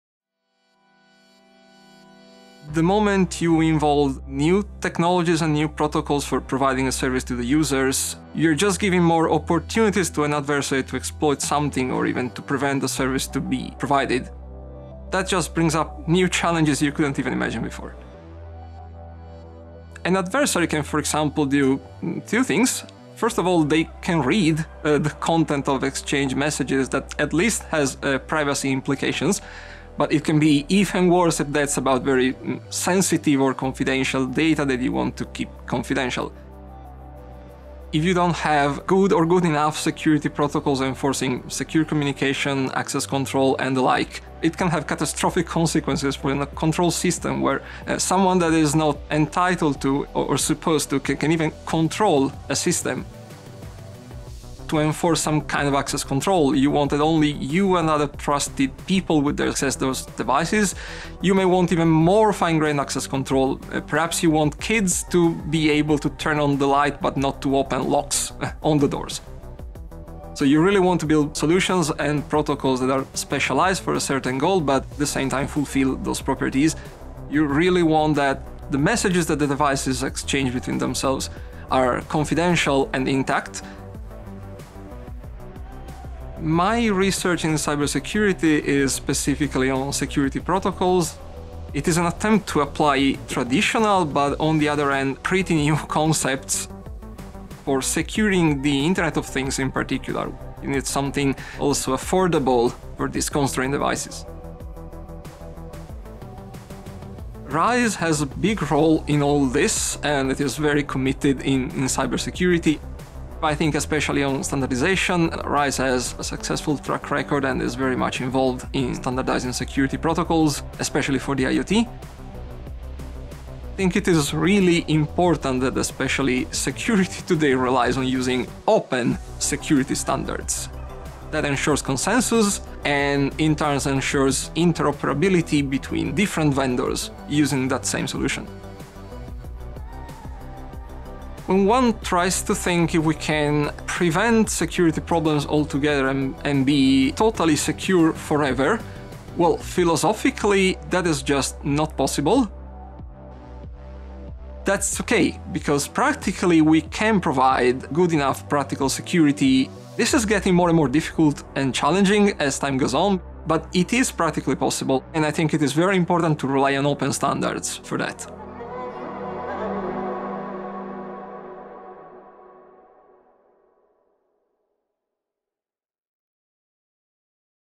Cybersecurity for Internet of Things (IoT)
With a growing number of internet-connected devices we need to ensure secure communication and access control.